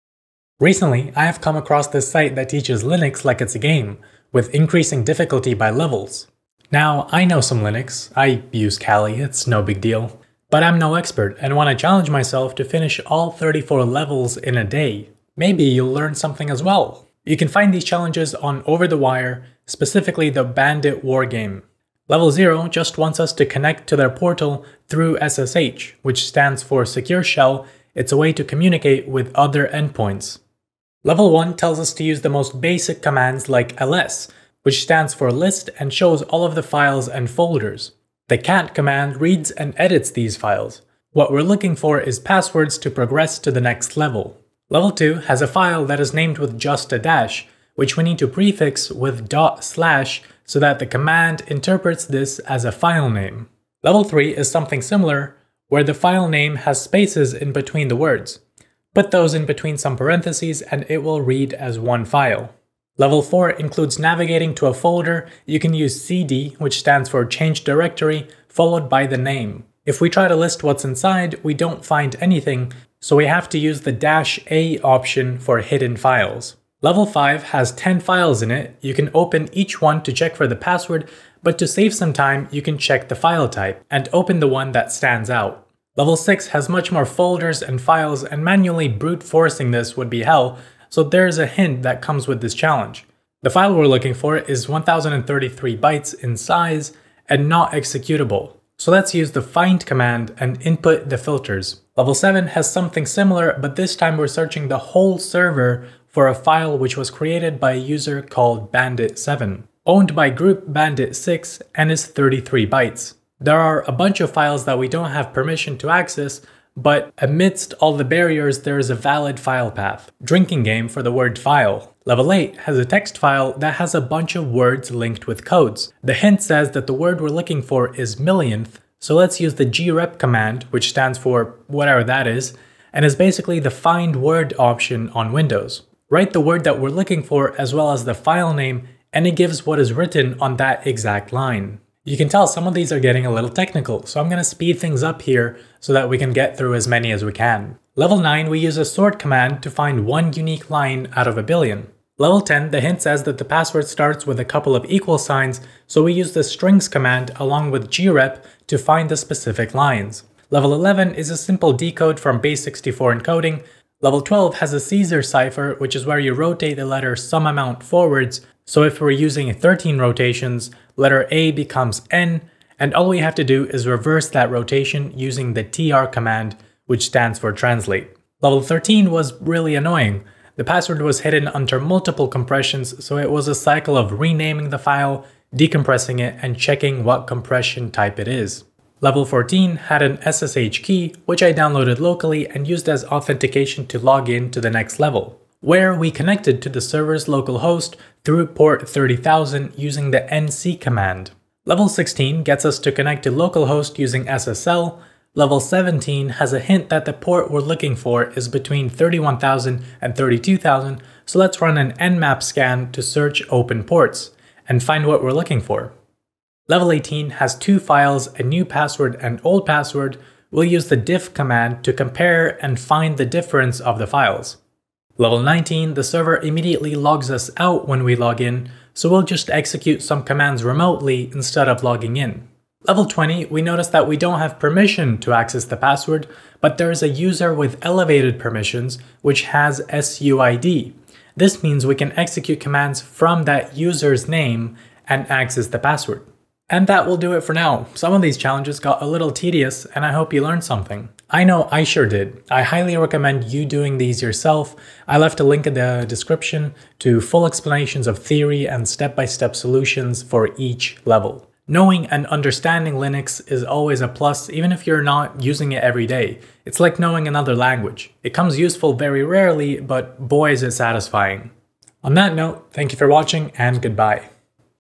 Speedrunning Linux (OverTheWire Bandit)
I just wanted to learn some linux commands to impress all the girls... let me know if you want to see the rest of the levels. A lot of the challenges included SSH, Linux find/sort/grep commands, using nmap and more.